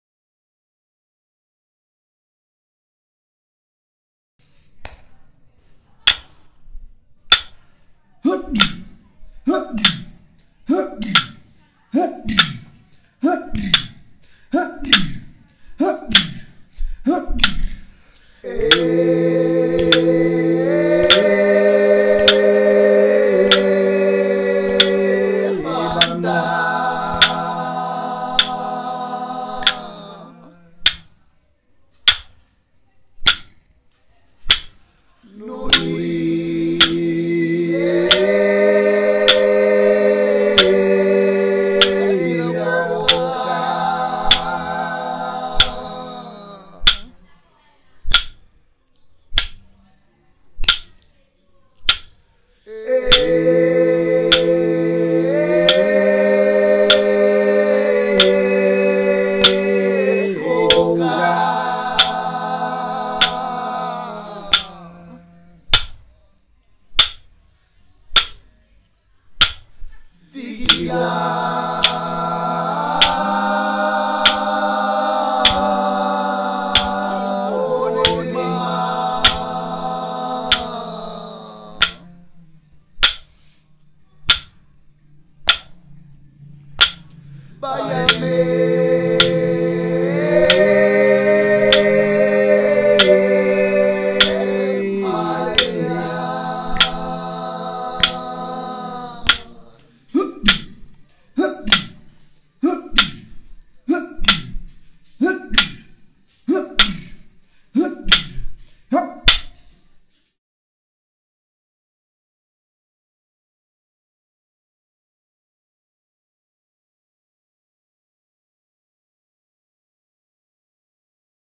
Aboriginal Song (My first coolaboration with my brother Jamie Mclennan)
A coolaboration with a great aboriginal musician, Jamie Mclennan. We met 2018 in Dubbo Australia